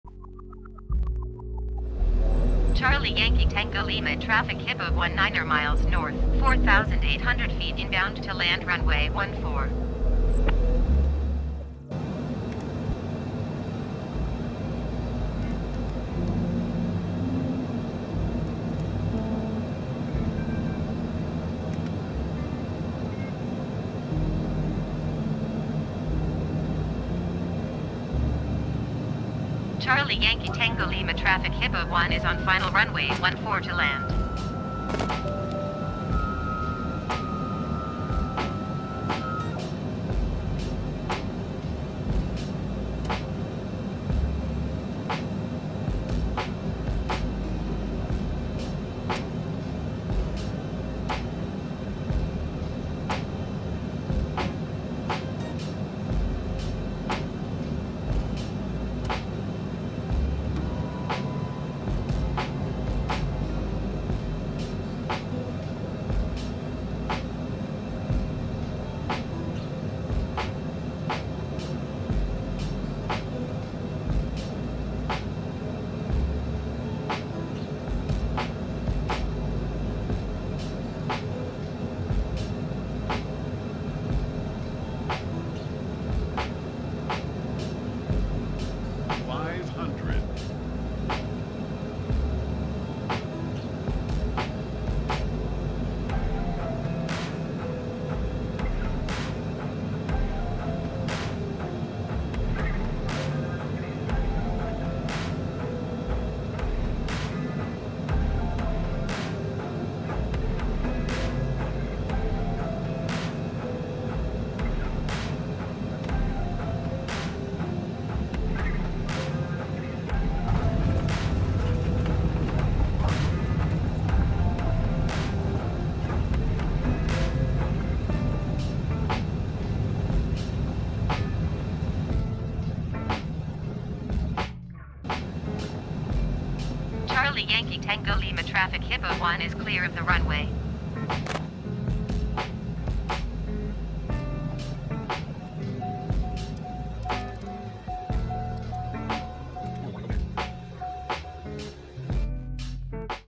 Cessna CJ4 Landing on Snow Covered Runway 14 at Big Trout Lake, Ontario, Canada | CYTL
Landing a Cessna CJ4 on Snow Covered Runway 14 at  Big Trout Lake, Ontario, Canada | CYTL